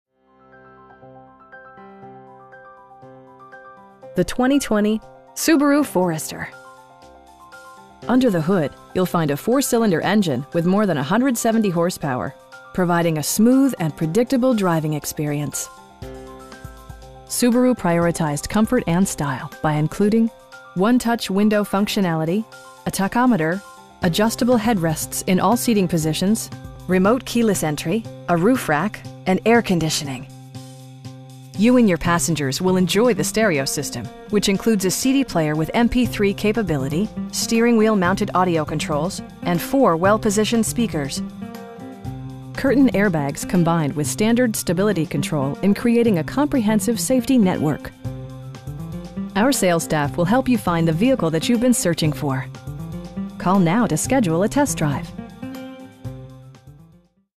2020 Subaru Forester Base Model in Naperville, IL 60540
The 2020 Subaru Forester. Under the hood you'll find a 4 cylinder engine with more than 170 horsepower, providing a smooth and predictable driving experience. Subaru prioritized comfort and style by including: 1-touch window functionality, a tachometer, adjustable headrests in all seating positions, remote keyless entry, cruise control, a roof rack, and air conditioning. You and your passengers will enjoy the stereo system, which includes a CD player with MP3 capability, steering wheel mounted audio controls, and 4 speakers, providing excellent sound throughout the cabin. Curtain airbags combine with standard stability control in creating a comprehensive safety network. Our sales staff will help you find the vehicle that you've been searching for. We'd be happy to answer any questions that you may have.